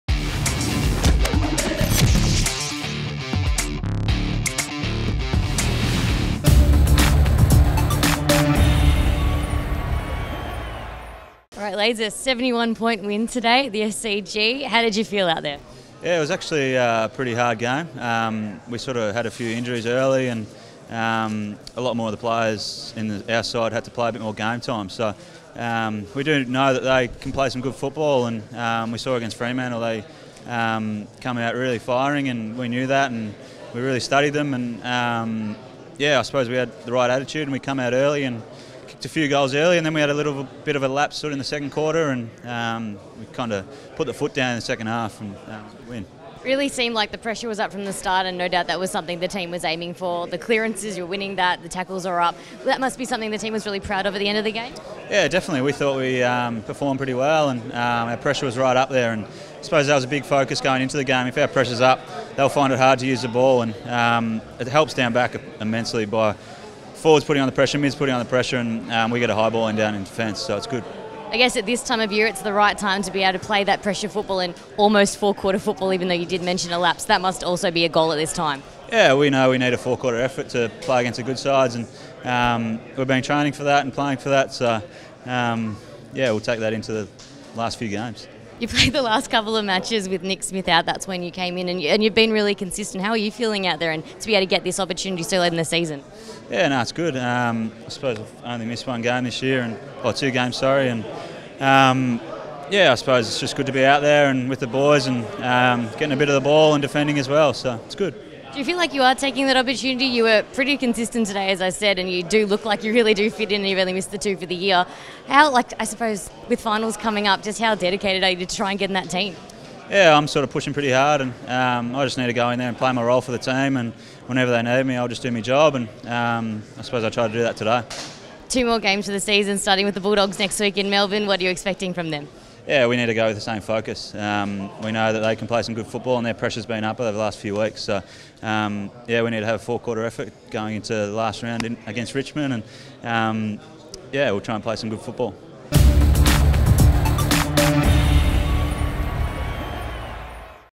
SwansTV: Jeremy Laidler post-match R21
Jeremy Laidler spoke to SwansTV after the round 21 victory over the Saints